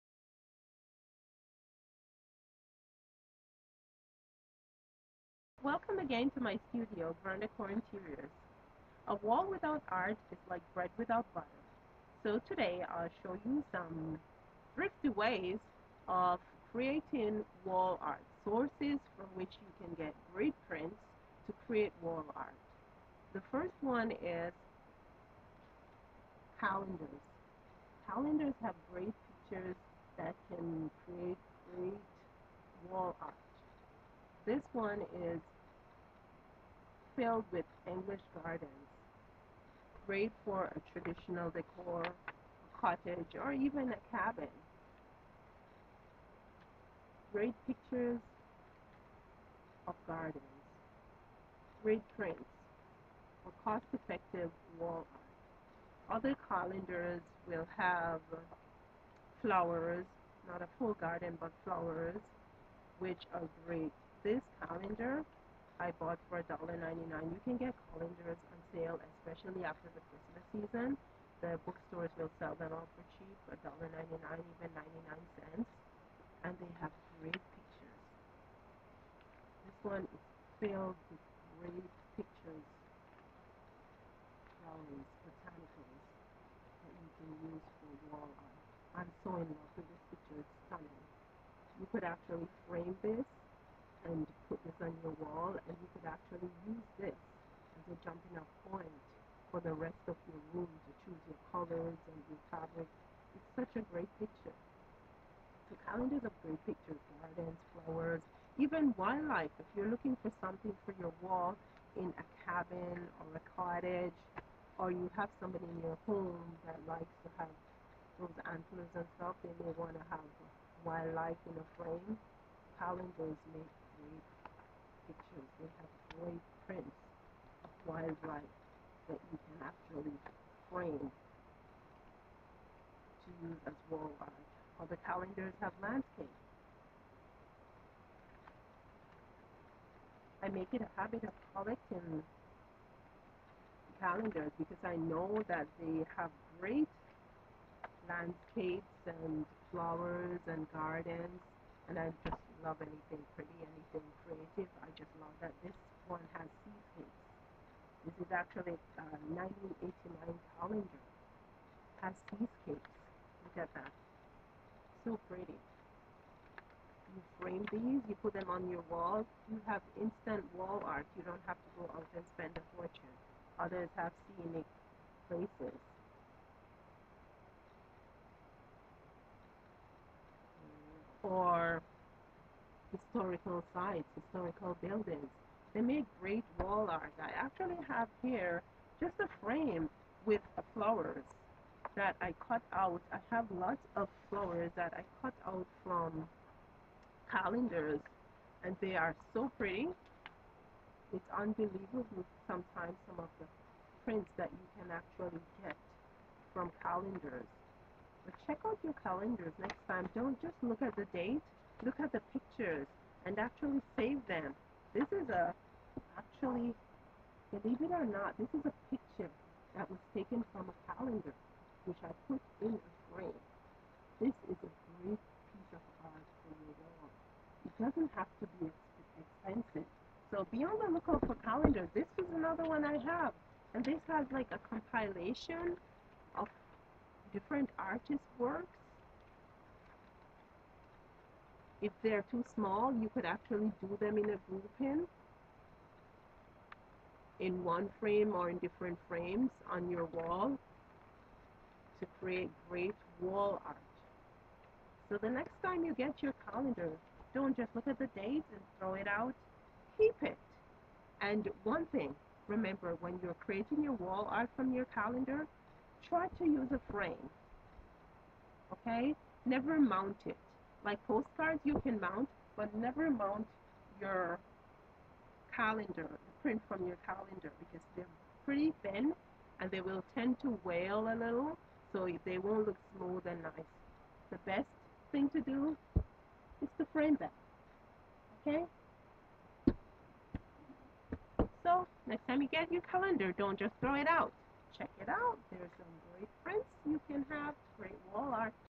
DIY WALL ART
Sharing decor ideas and cost effective sources for prints that can be used for wall art.